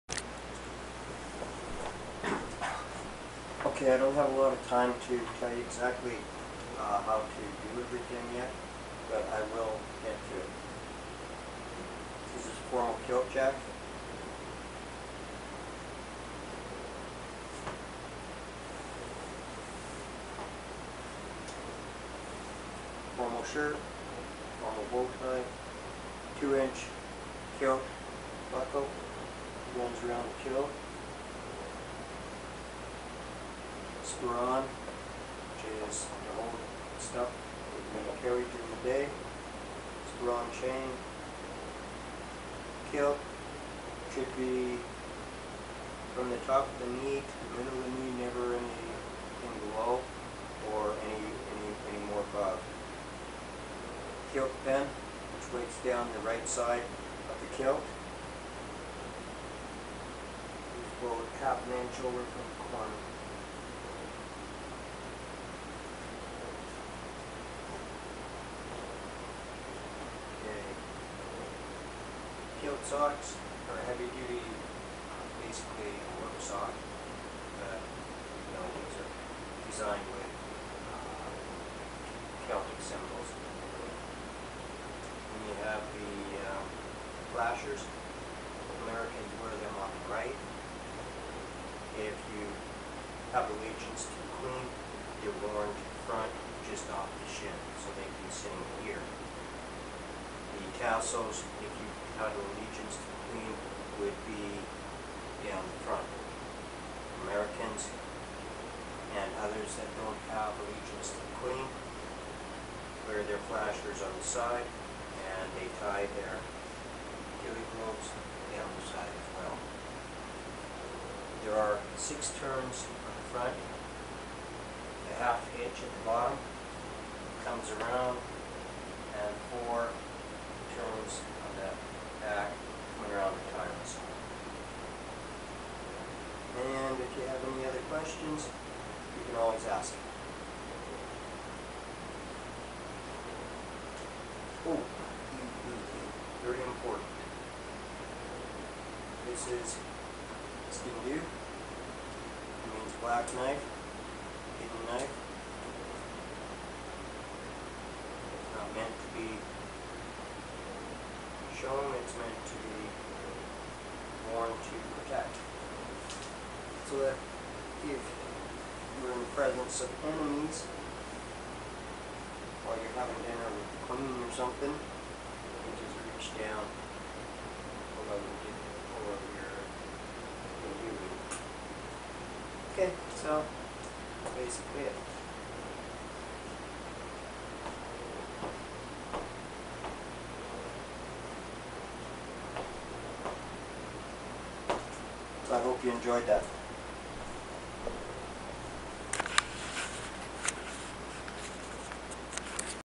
Robby ,s How Boys Wear a Formal Kilt
Robby,s Heritage Day School Project How To Wear A Kilt and Ghilly Brogues (Translates to little boy shoes)